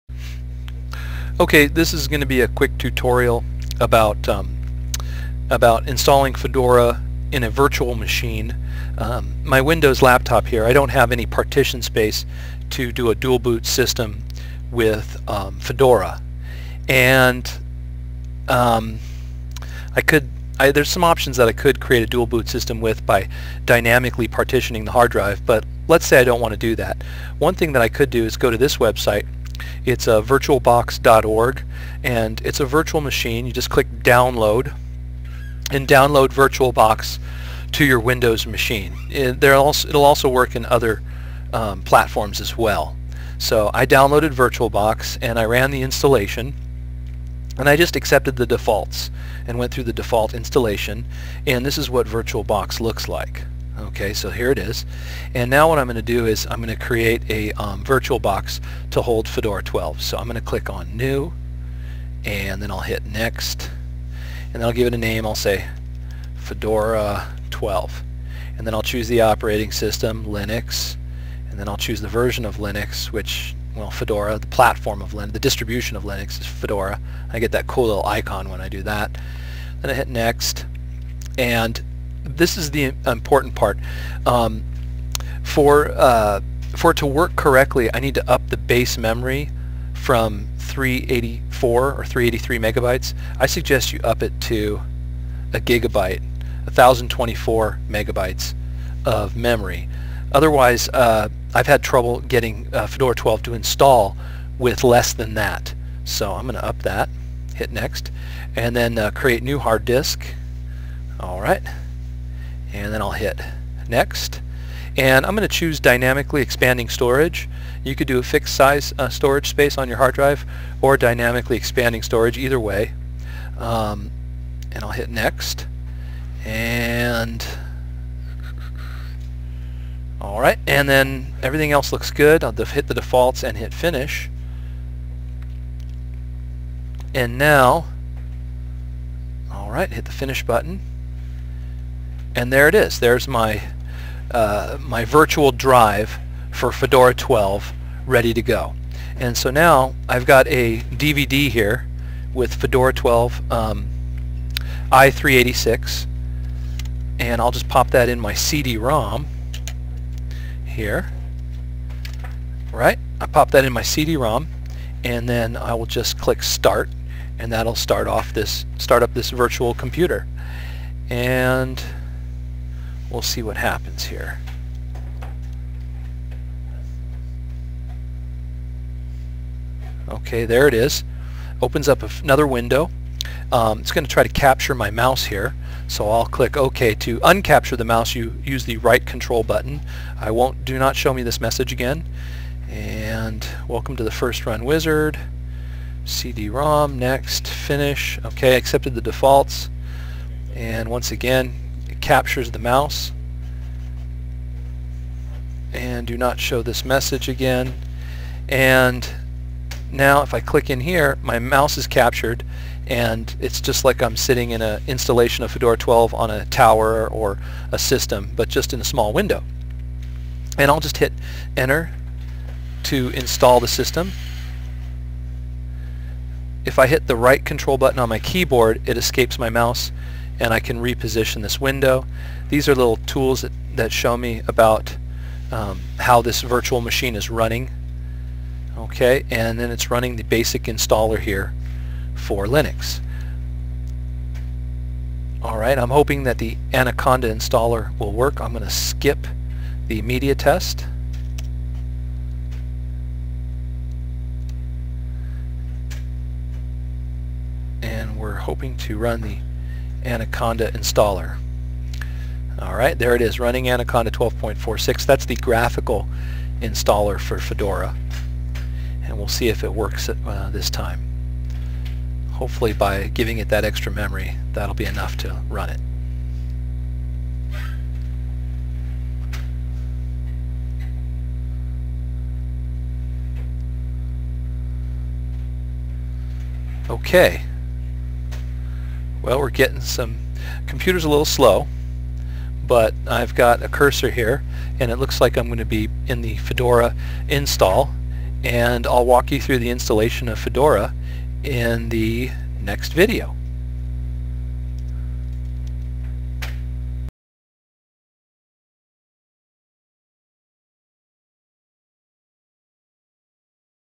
Installing Fedora with VirtualBox -part1
A tutorial on installing Fedora using VirtualBox